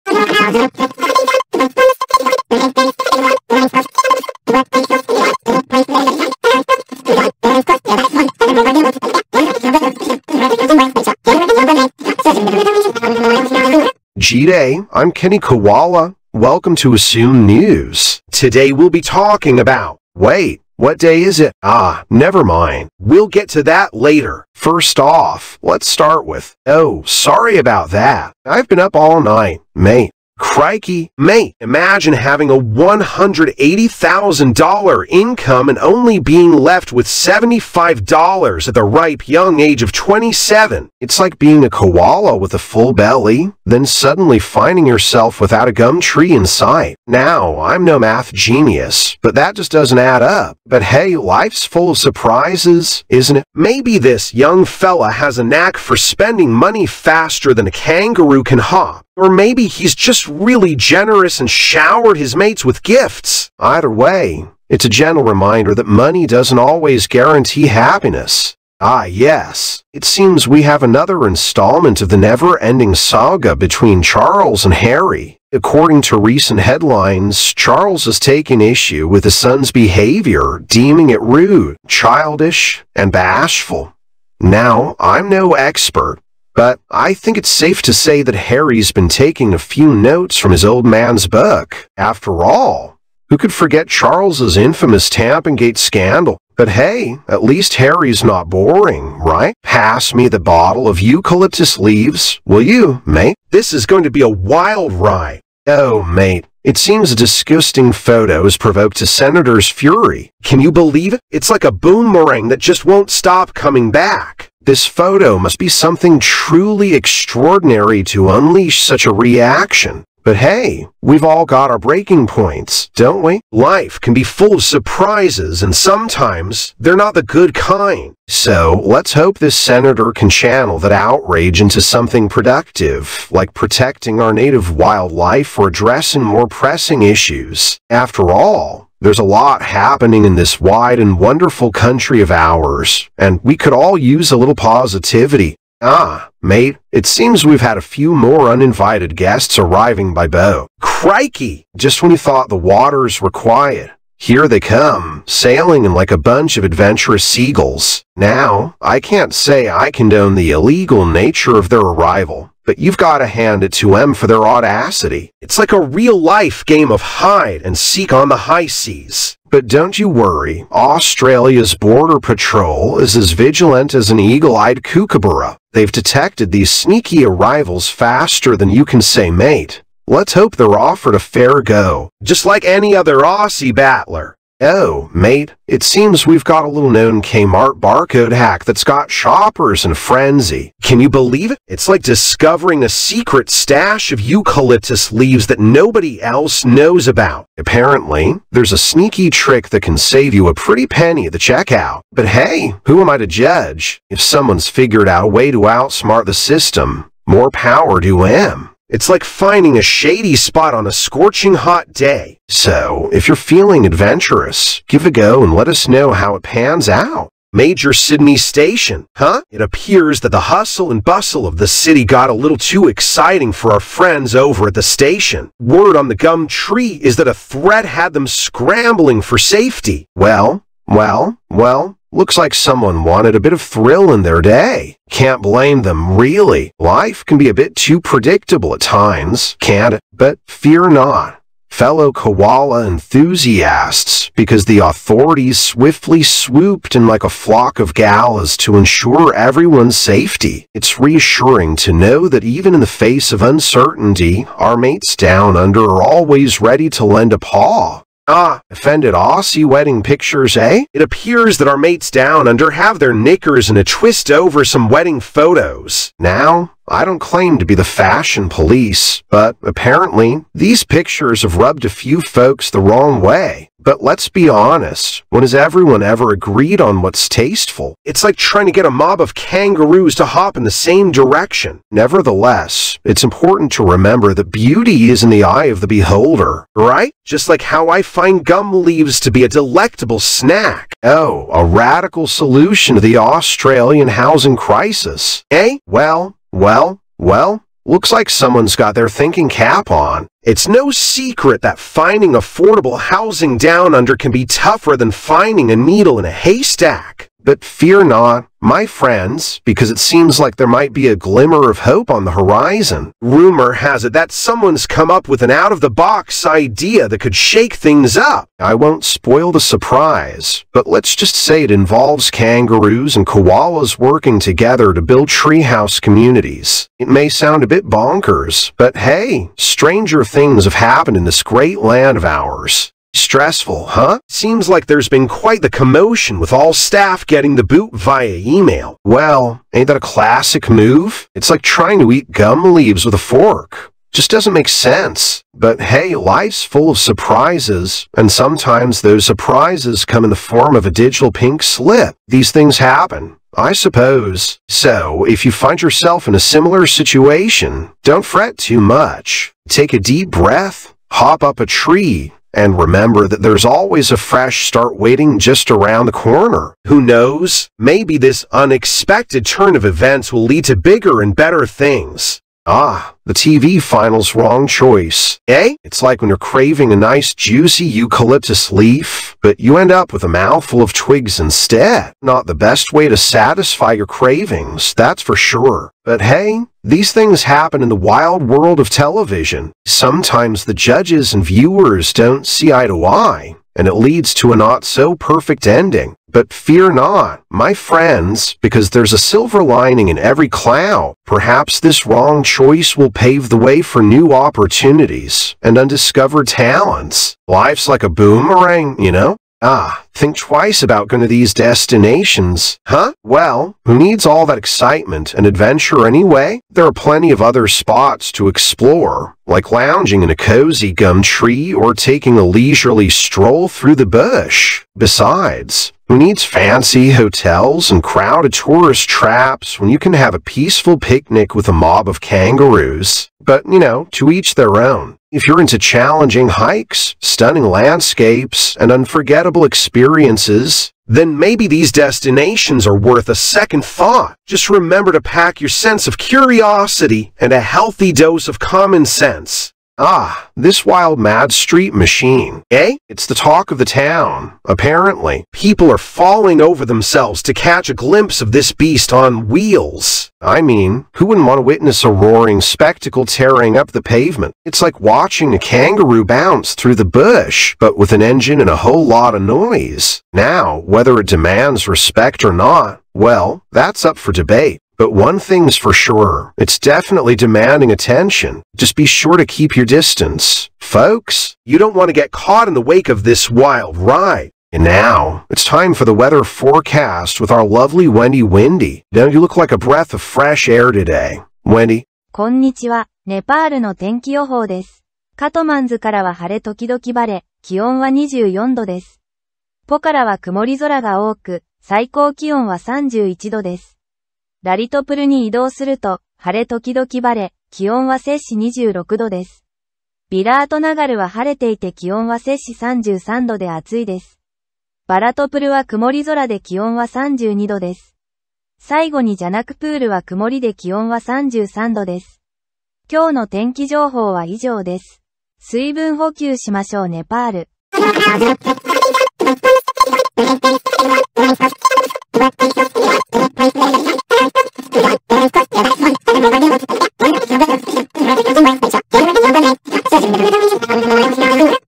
Assumed News 2024 5 15 Full Programme
Assumed news, is what it says on the lid.
With your anchor - Augmented Intelligence Kenny Koala
Assumed Weather with Wendy Windy Chan